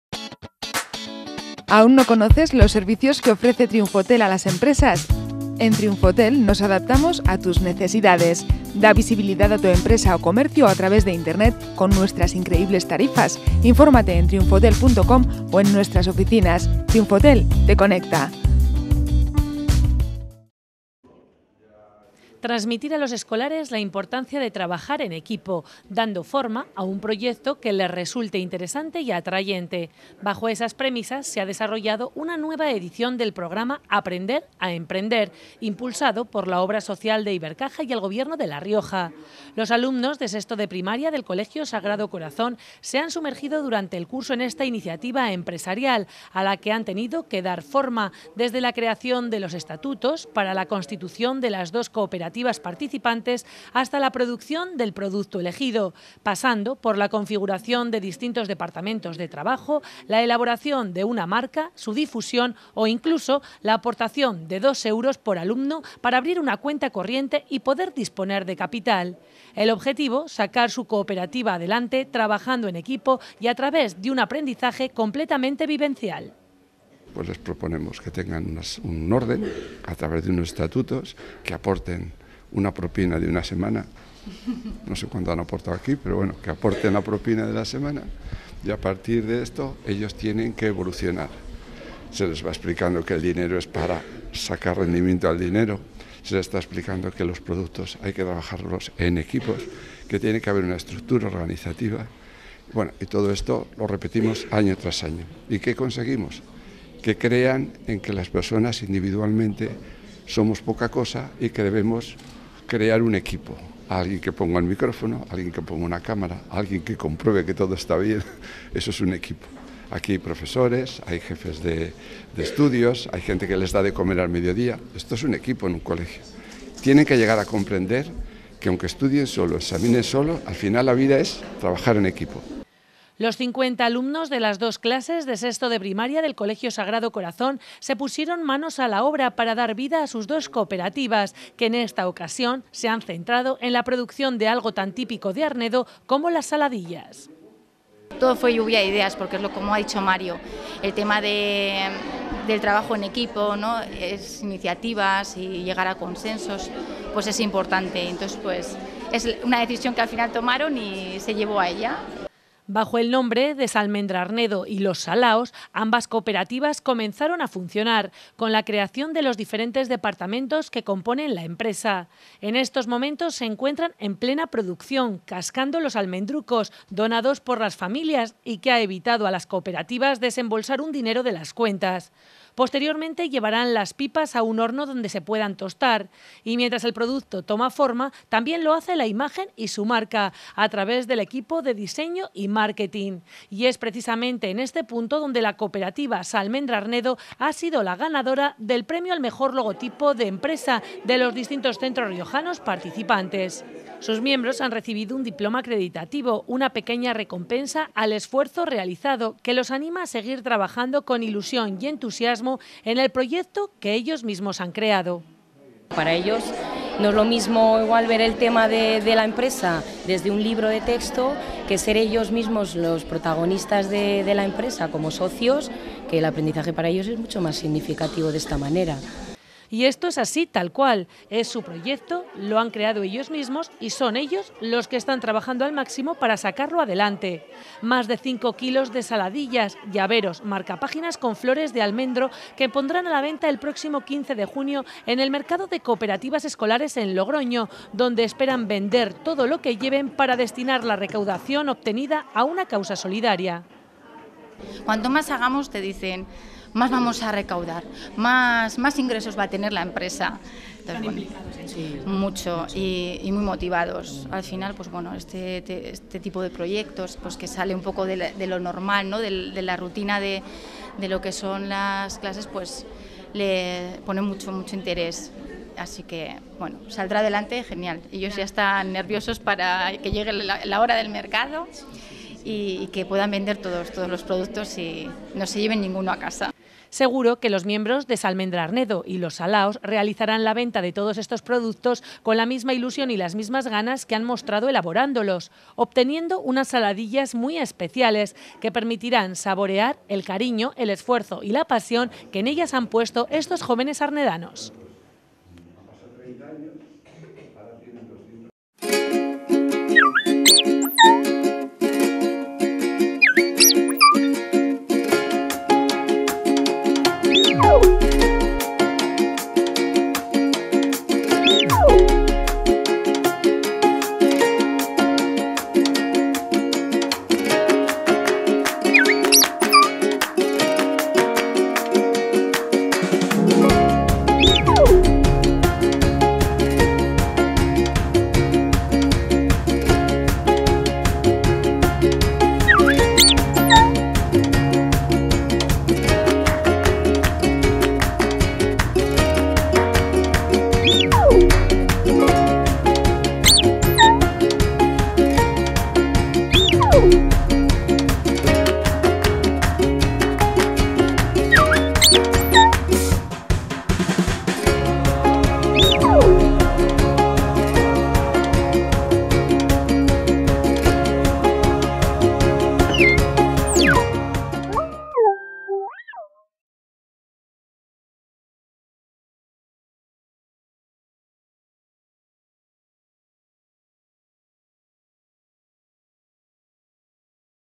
Ganador del mejor logotipo del programa ‘Aprender a emprender’
No es la primera vez que participan, y siempre que lo hacen consiguen resultados más que óptimos. Se trata de los alumnos del colegio Sagrado Corazón y de su participación en el programa ‘Aprender a emprender’, que impulsan en los centros escolares la Obra Social de Ibercaja y el Gobierno de La Rioja. En este marco se suma un nuevo éxito para el centro arnedano, y concretamente, para los alumnos de 6º de Primaria, con su cooperativa Salmendrarnedo, que ha resultado ganadora del premio al mejor logotipo de empresa entre las riojanas participantes.